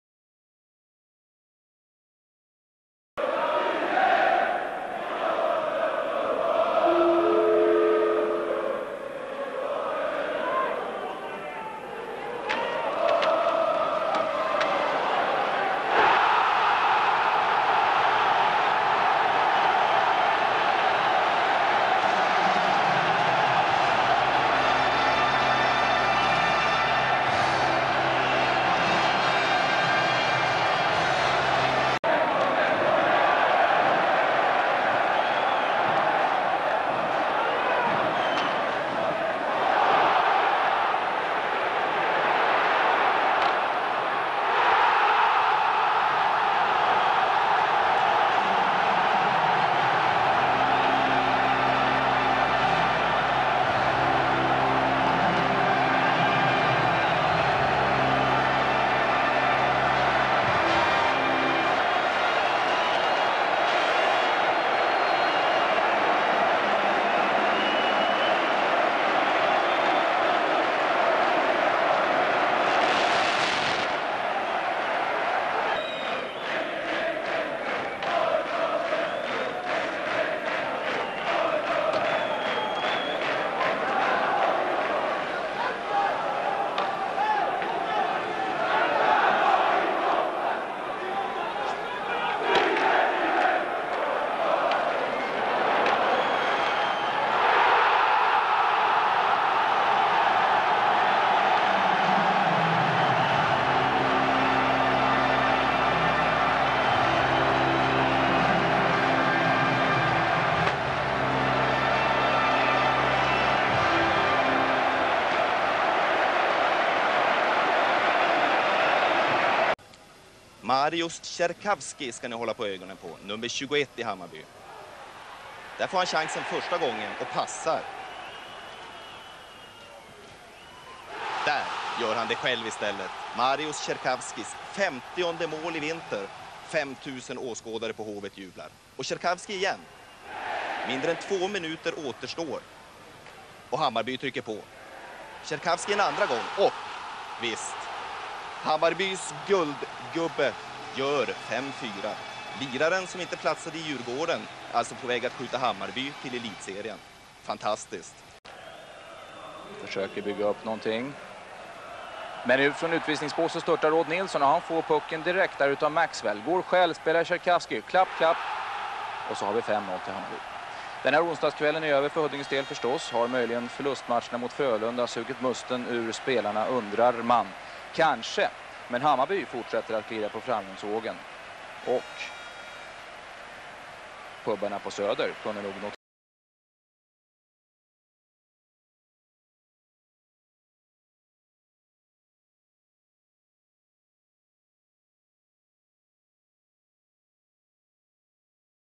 Mariusz Czerkawski Hammarby IF 1993
Kilka bramek Mariusza Czerkawskiego z 1993 roku w barwach Hammarby IF ze szwedzkiej Division 1. Polski napastnik w fazie play-off trafiał do bramki: AIK Sztokholm (12 256 widzów!), Troja/Ljungby oraz Huddinge.

Czerkawski ustanowił rekord strzelecki i w klasyfikacji kanadyjskiej w lidze Division 1 (obecnie Allsvenskan)  - 92 punkty (w tym 55 goli) w 45 meczach.